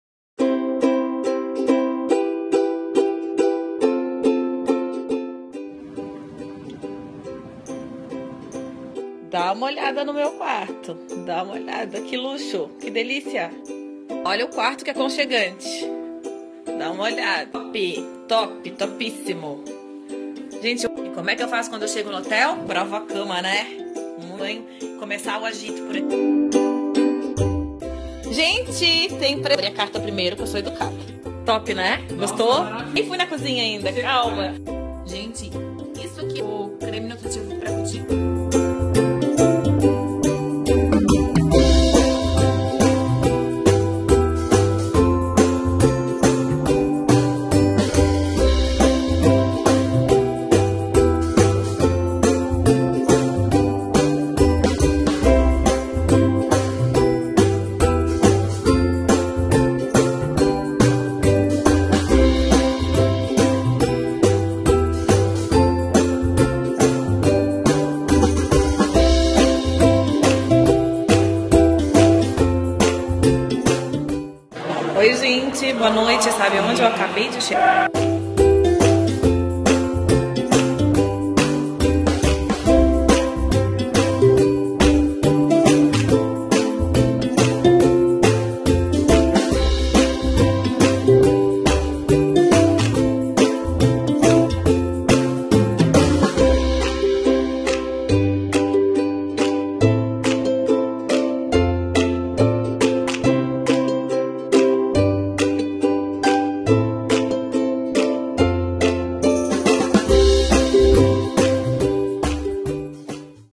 CFVIAJA em SÃO PAULO
Assista o making off dos meus dias em São Paulo, com direito a uma super produção e alguns famosos!